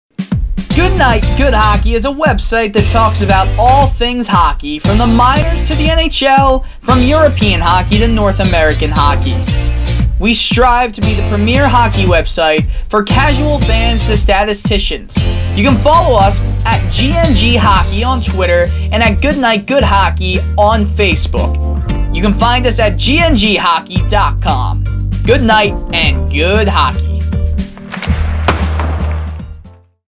Good Night, Good Hockey Trailer
Good Night, Good Hockey is a website that talks about all things hockey, from the minors to the NHL, from European hockey to North American hockey. We strive to be the premier hockey website for casual fans to statisticians. You can find us at gnghockey.com.